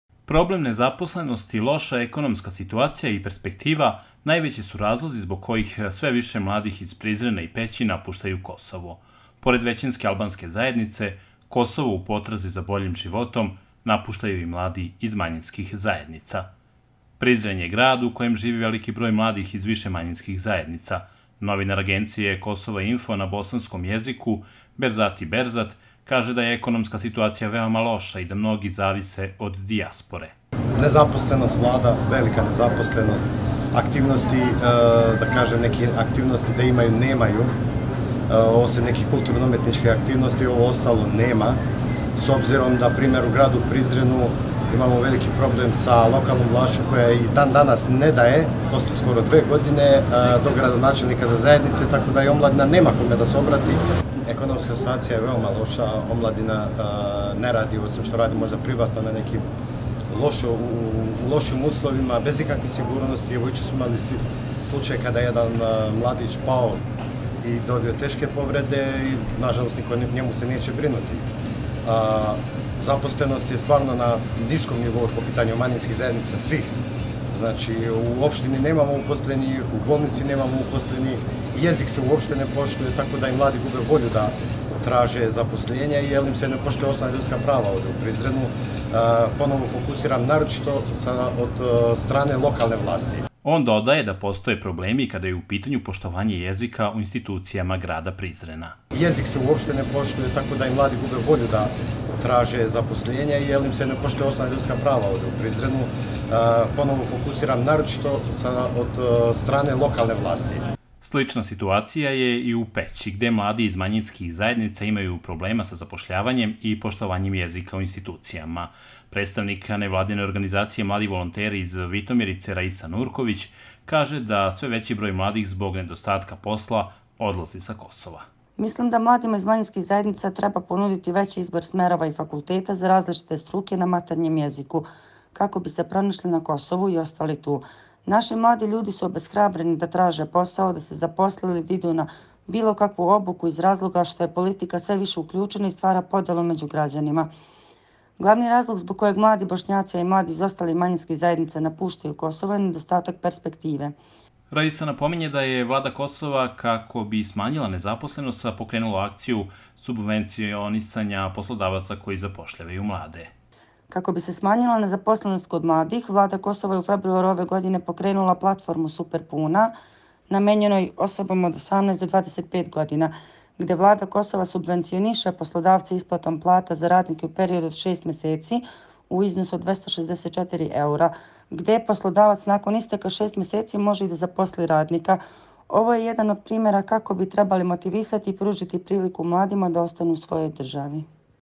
Nezaposlenost i poštovanje jezičkih prava jedni od problema sa kojima se suočavaju manjine na Kosovu
Problem nezaposlenosti, loša ekonomska situacija i prespektiva najveći su razlozi zbog kojih sve više mladih iz Prizrena i Peći napuštaju Kosovo. Pored većinske albanske zajednice, Kosovo u potrazi za boljim životom napuštaju i mladi iz manjiskih zajednica.